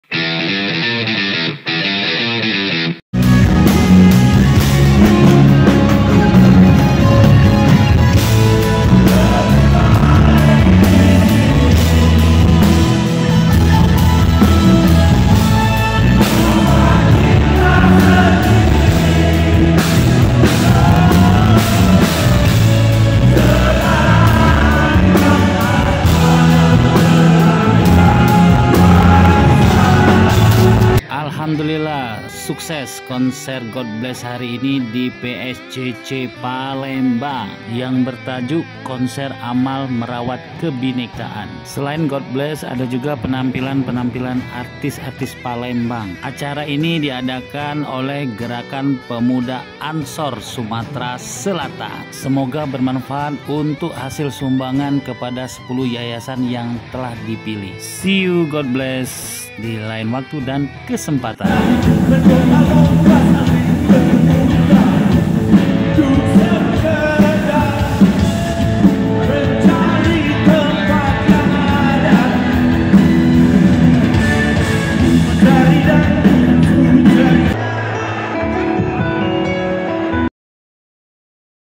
Sukses Konser GODBLESS di PSCC Palembang yang di adakan oleh Gerakan Pemuda Ansor Sumatera Selatan.
Sukses Konser GODBLESS hari ini 20 Juli 2024 di PSCC Palembang yang di adakan oleh Gerakan Pemuda Ansor Sumatera Selatan.